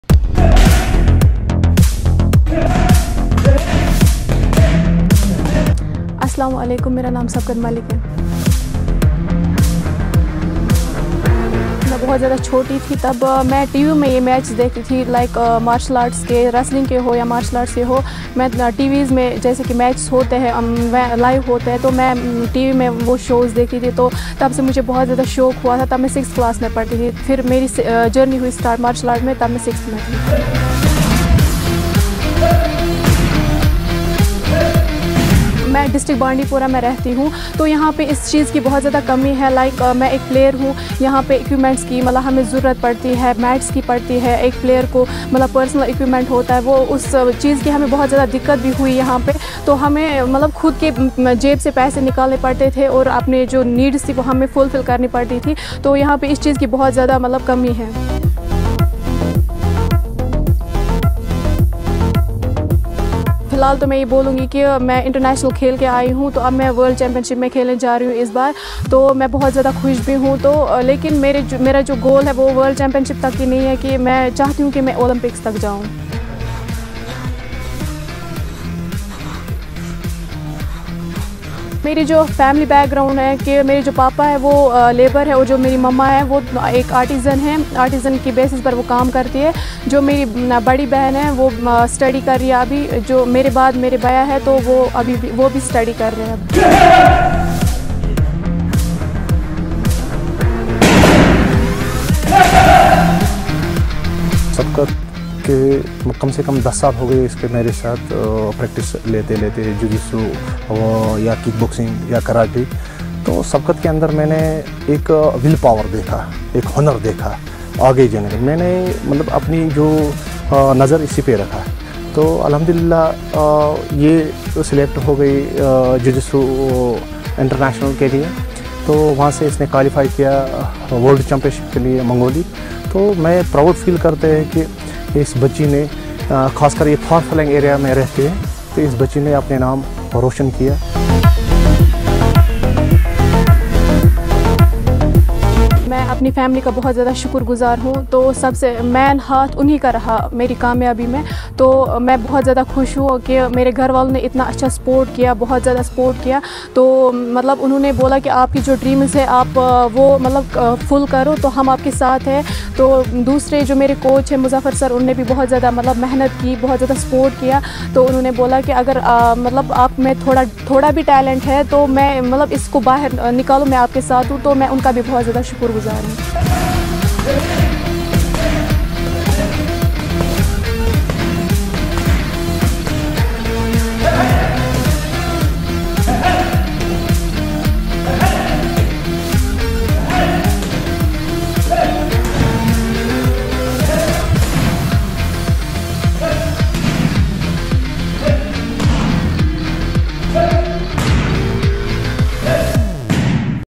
Sabqat Malik from Kashmir to represent India In Martial Arts at World Championship | Oneindia News
Sabqat Malik is a 20-year-old girl from Kashmir. She was passionate about Martial Arts when she was just a child studying in grade 6th. She shared her journey coming from a small town to reaching heights as she will be representing India In world Championship with Our team Exclusively. Watch it and feel proud of her as we are to share her story to you.